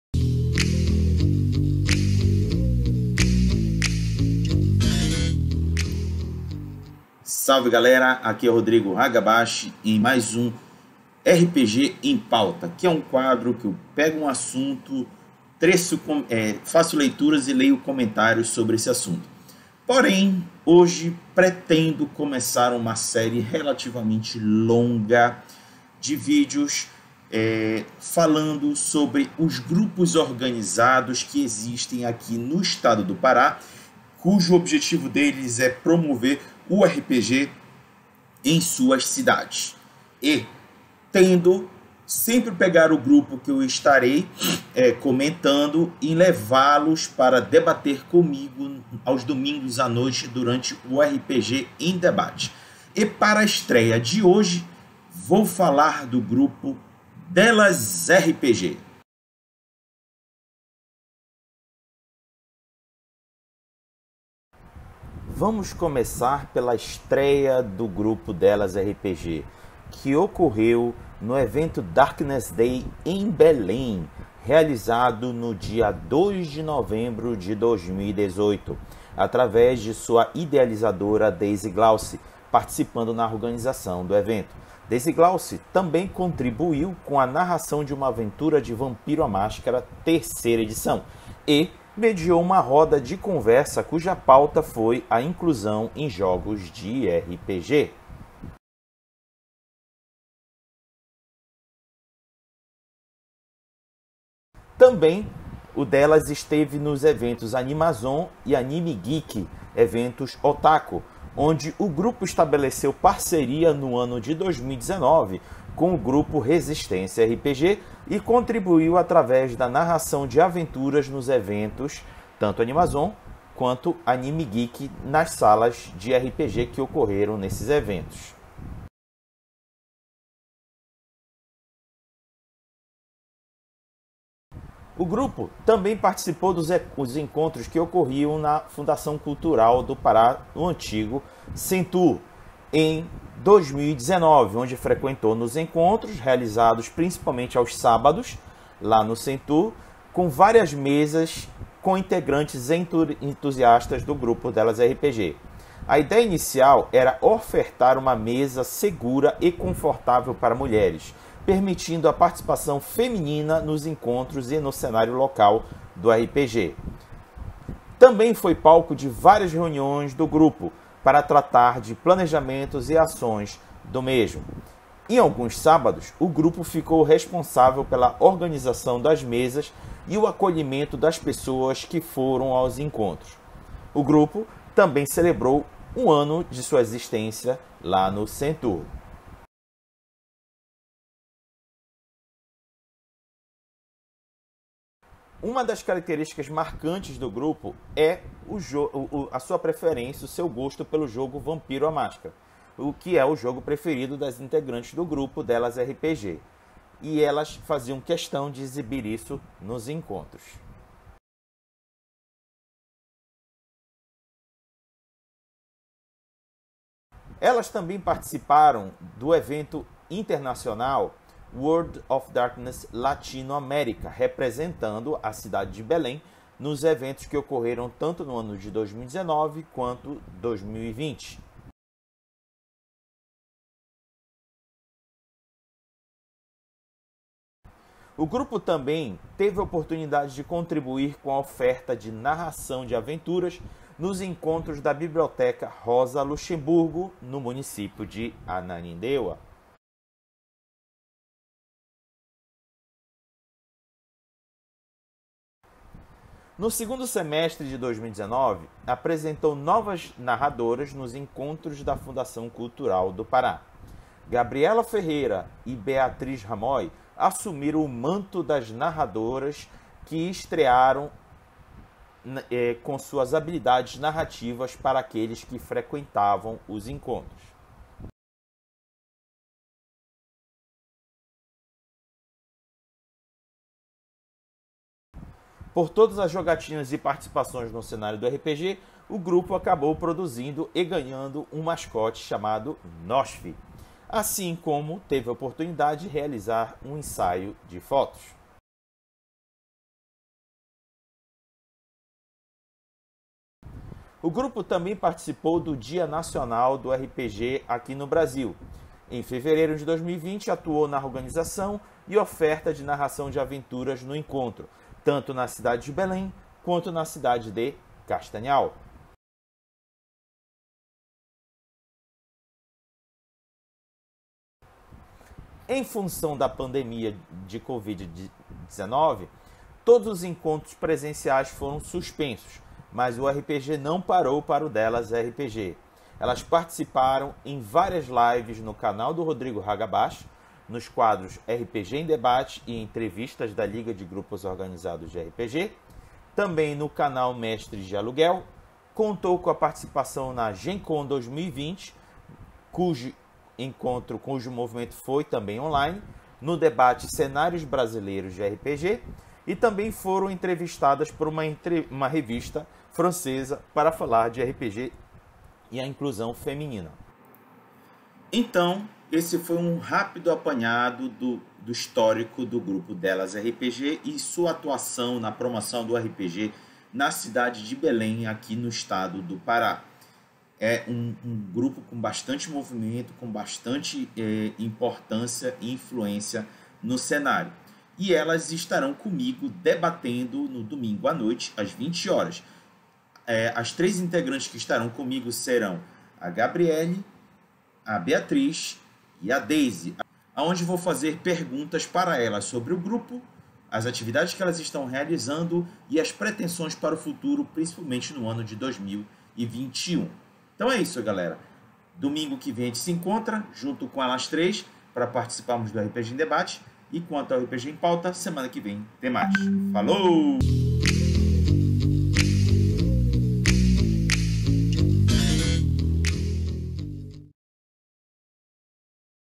RPG em Pauta: Grupo Delas RPG
O RPG em Pauta é um quadro em que irei comentar sobre os últimos acontecimentos do cenário do RPG, principalmente os do Estado do Pará.

Loja virtual de livros, suplementos e vestuário de RPG do Raga